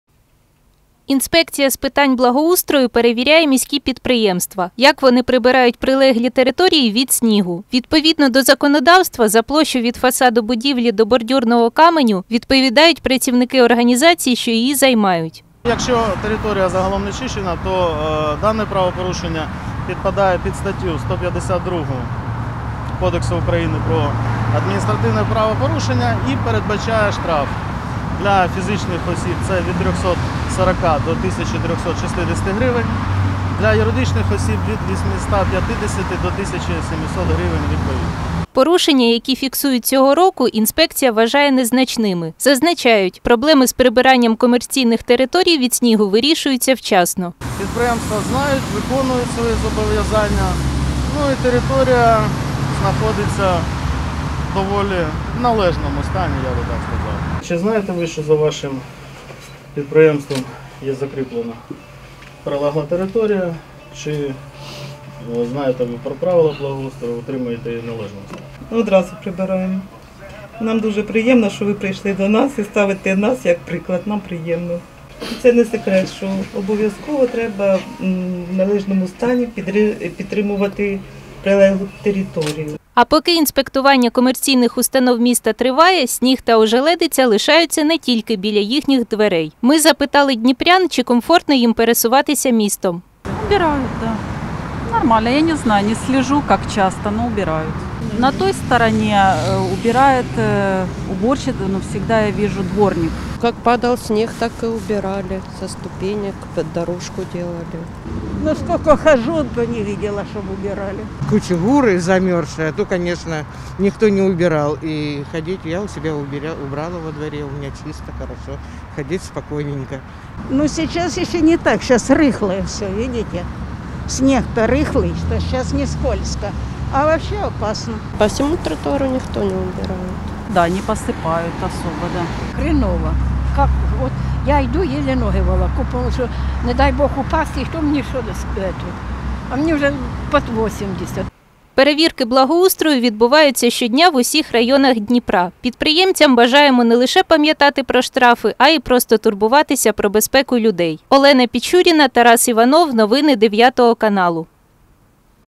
Несвоєчасне прибирання снігу на прилеглій території загрожує штрафом
Інспекція з питань благоустрою розпочала традиційні перевірки - дивиться, як підприємства та комерційні заклади прибирають прилеглу територію від снігу.